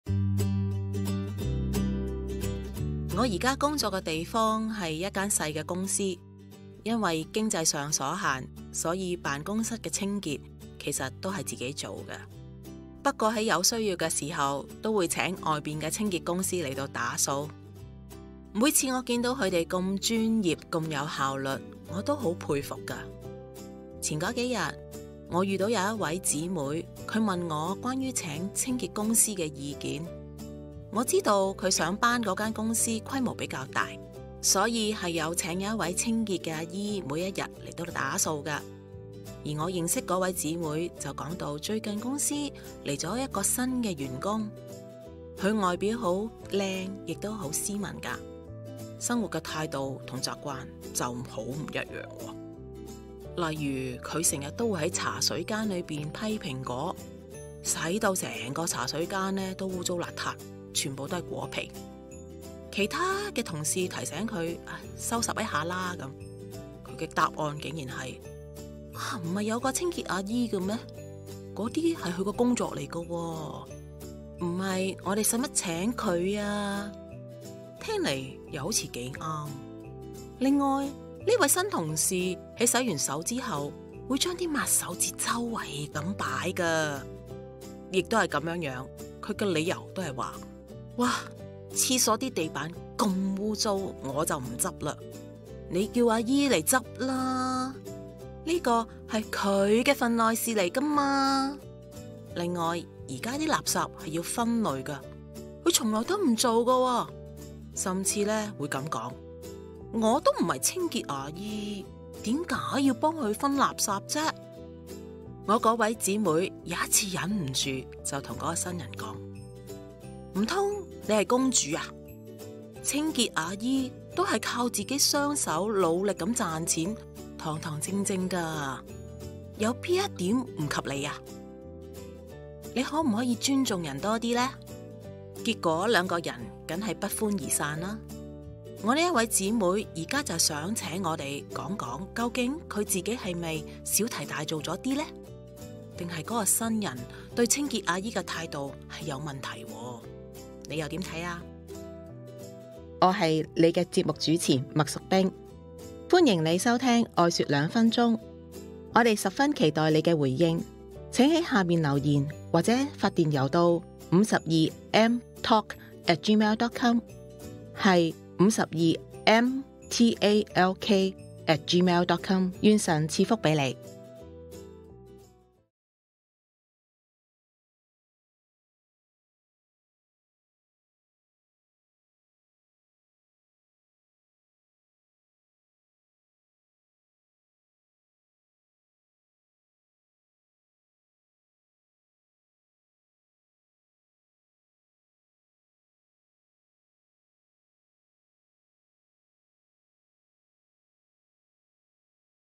新人與清潔阿姨
維護公共環境衛生，也是一種對人的尊重。

愛說兩分鐘（粵語）
Episode 135